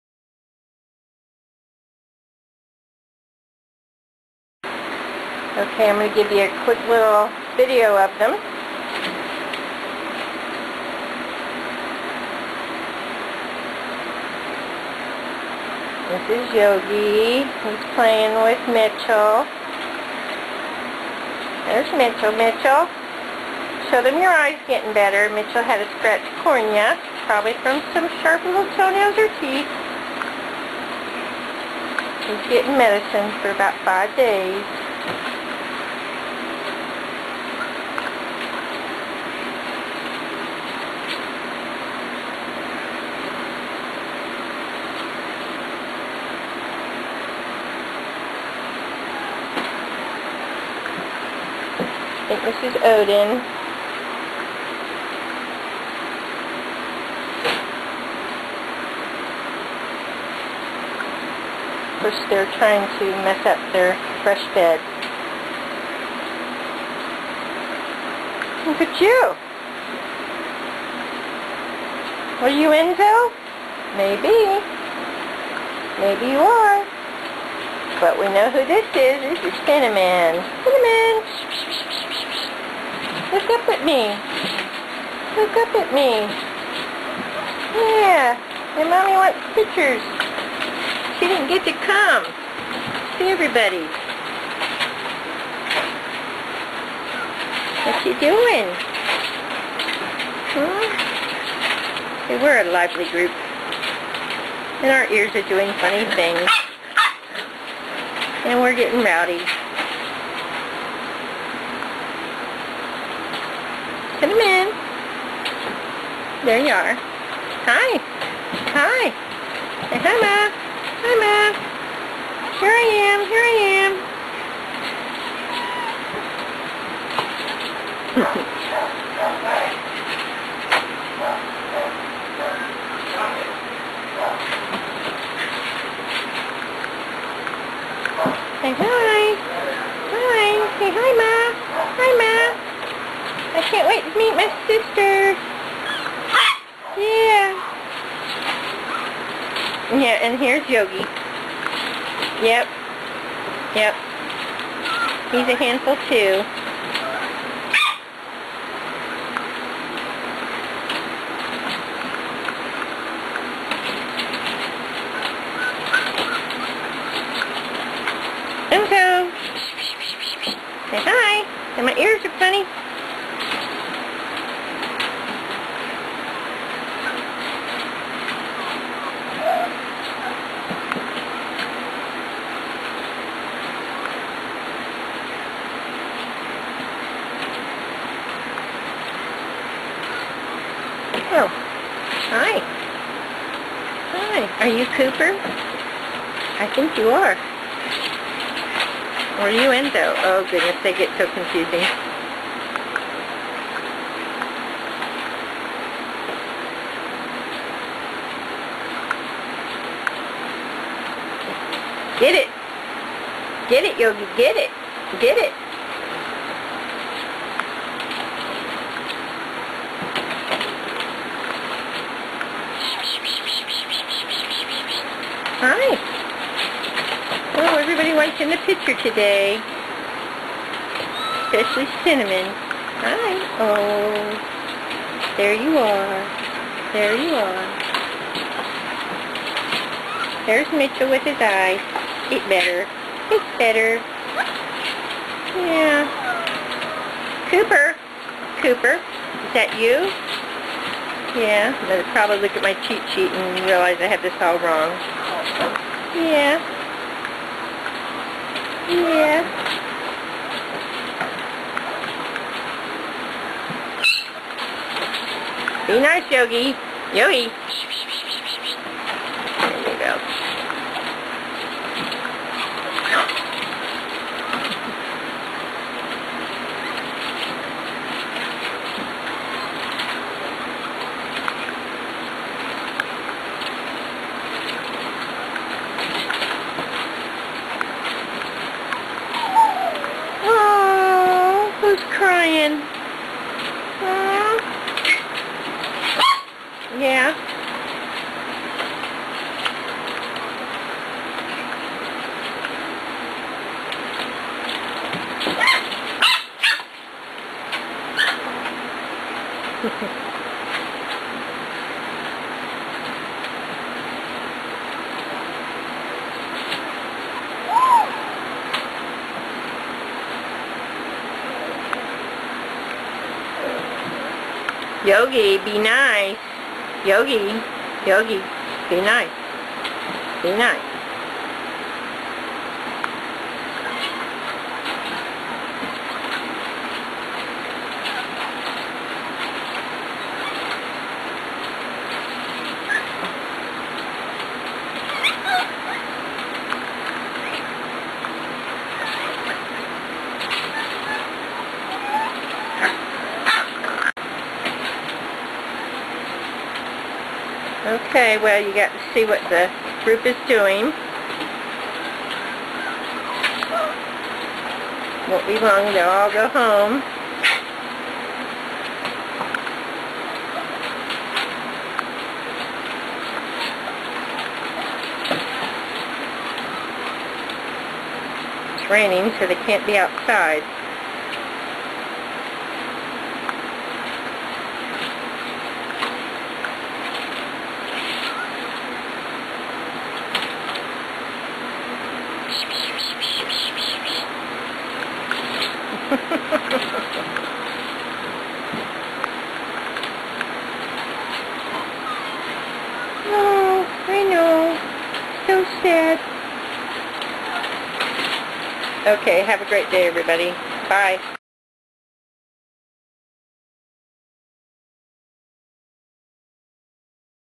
Corgi puppies
Zoe and Little Man Corgi Puppies 10/30/09
They are about 7 weeks old.